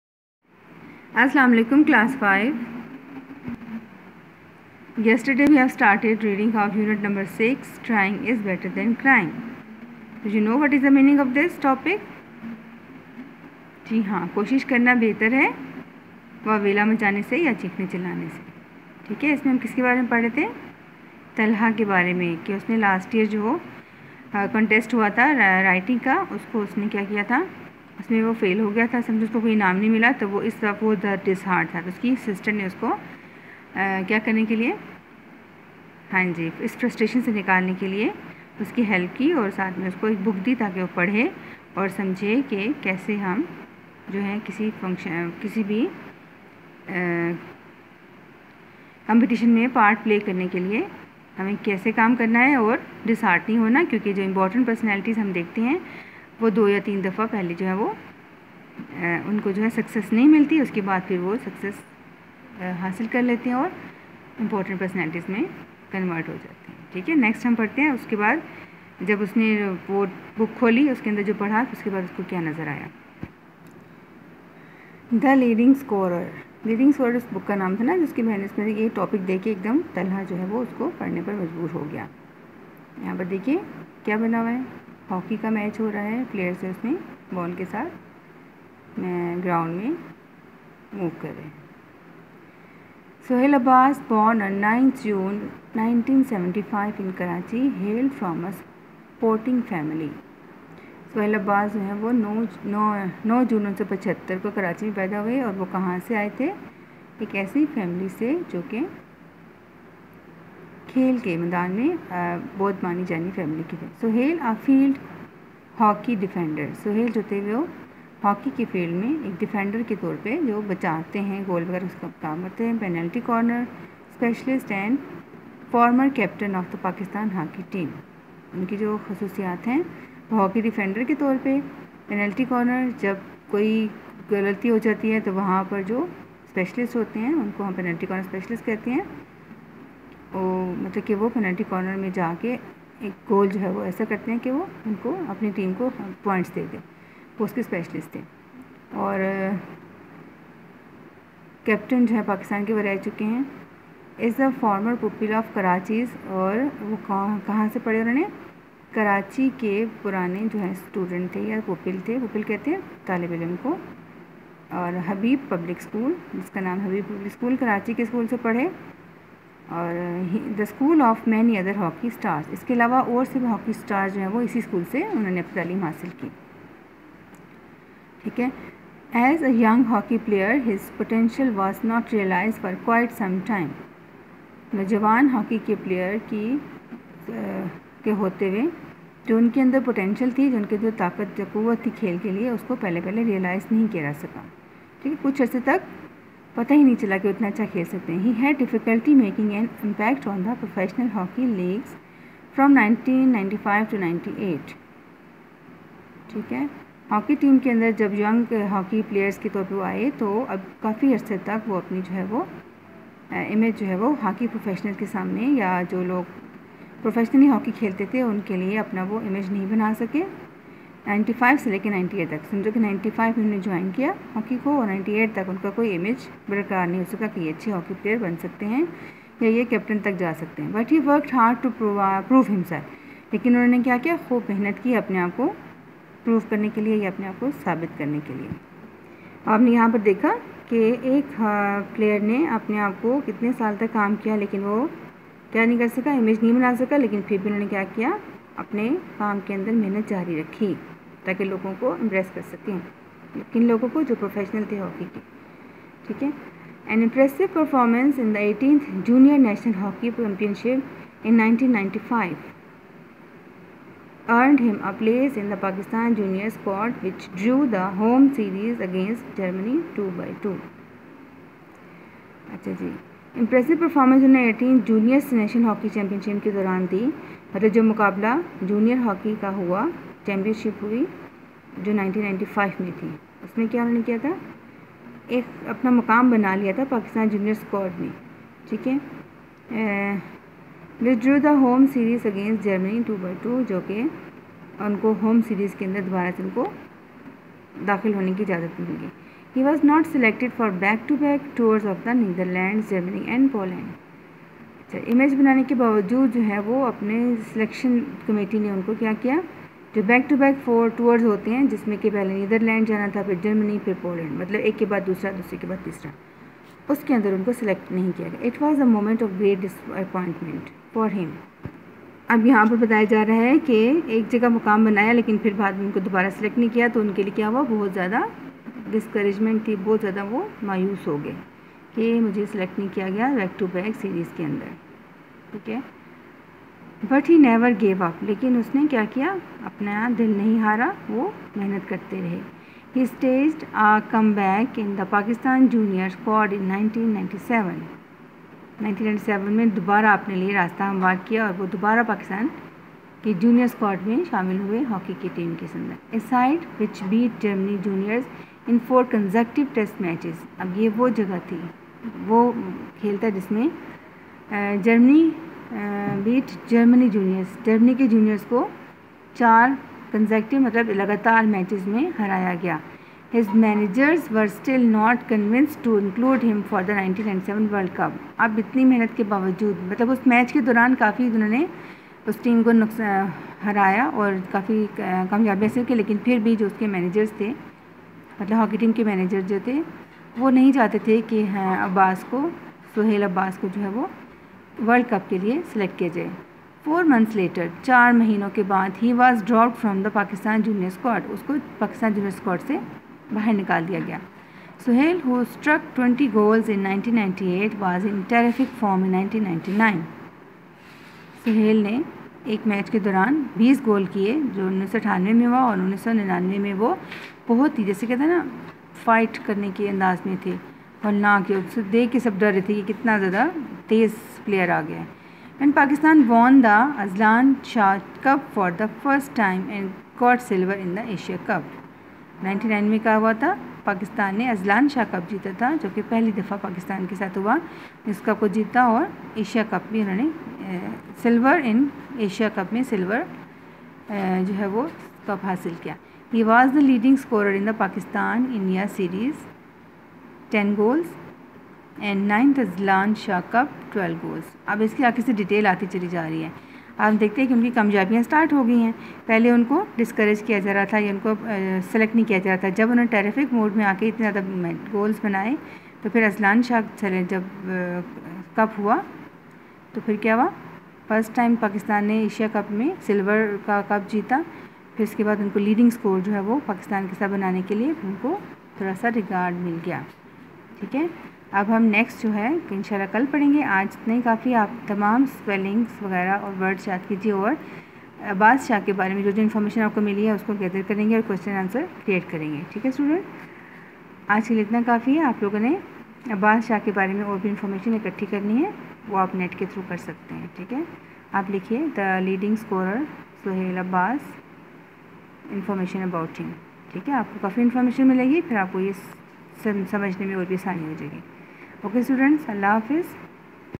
Class 5th | Englisg | Lesson no 6 | Trying is better than Crying | Online Class
English Class 5th. Unit6.Trying is better than Crying. Reading and explaination.H.W. Creative writing (make question answers learn spellings for dictation. )